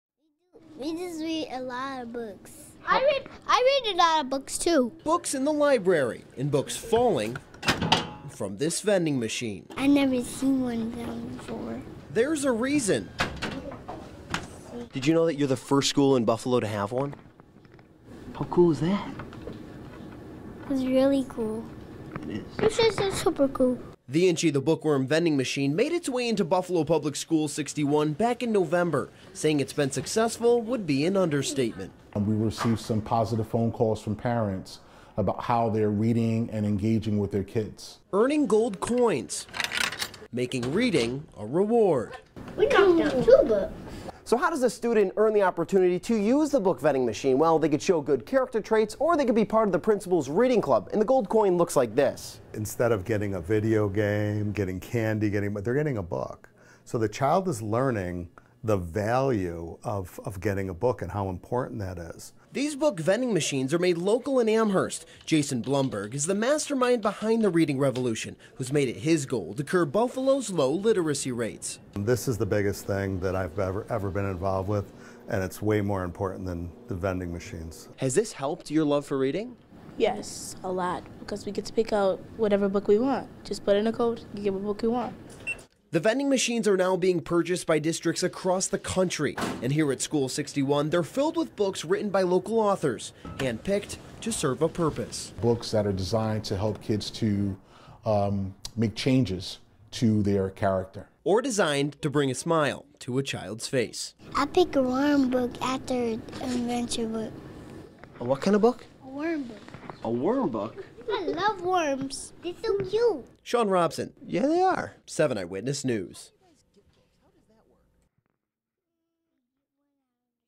Books in a vending machine? A reading revolution.
Normally, snacks and soda are things you'll find in a customary vending machine... but not the vending machine inside Buffalo Public School 61.